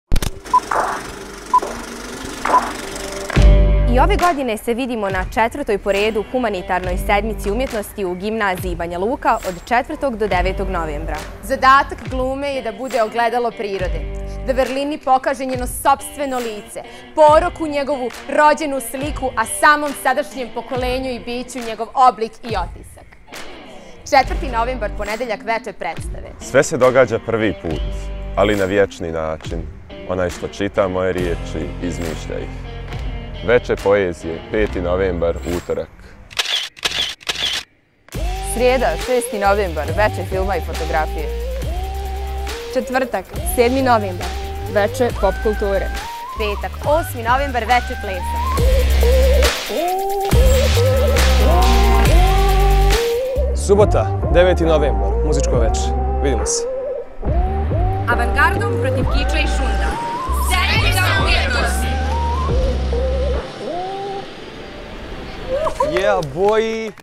Sedmica Umjetnosti 2019 Gimnazija Banja Luka
Gimnazijalci i ove godine organizuju humanitarnu manifestaciju na kojoj će sakupljati novac za plaćanje ekskurzije učenicima Gimnazije u Banjoj Luci, koji su u lošoj materijalnoj situaciji i ne mogu da je plate.